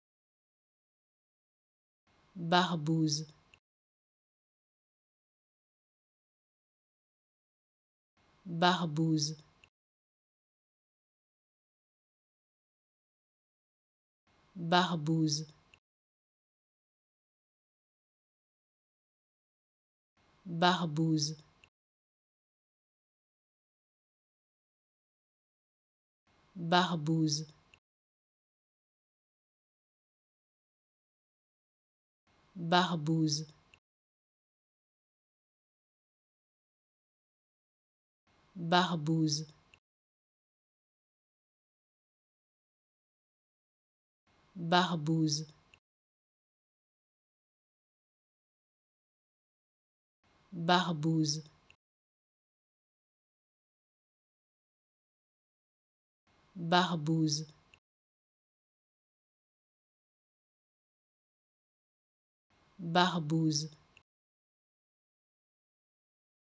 How to Pronounce ''Barbouze'' Correctly in French
Learn how to say and properly pronounce ''Barbouze'' in French with this free pronunciation tutorial. Apprenez à prononcer des mots en français grâce à nos tutoriels.